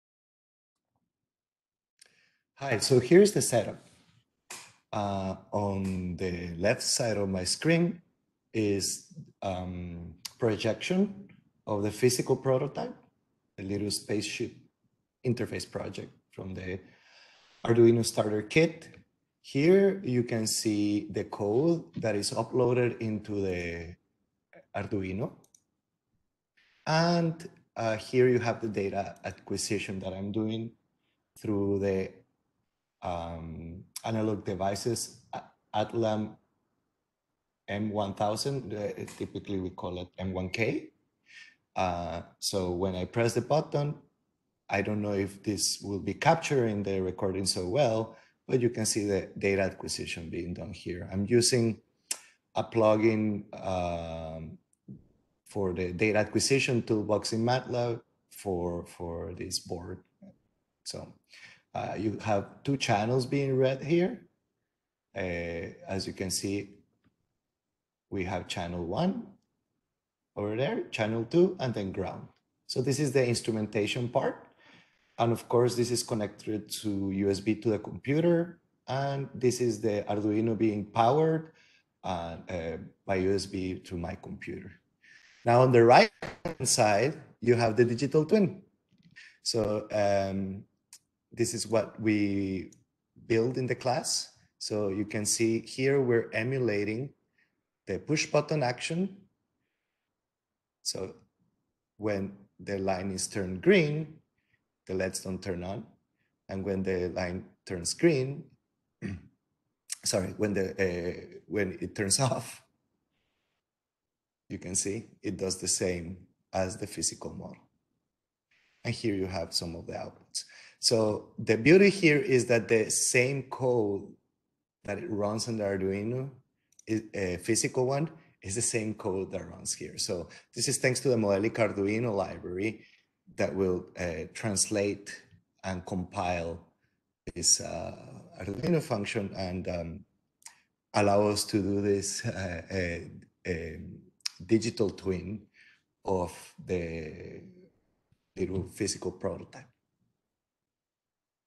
Luigi's Lab 1 Setup Demo - Digital Twin of the Arduino "Spaceship Interface" Project using Modelica
This is a demo of my setup for one of the labs carried out in my course ECSE 4170/6170 "Modeling and Simulation of Cyber Physical Systems" thought during Spring 2021 remotely.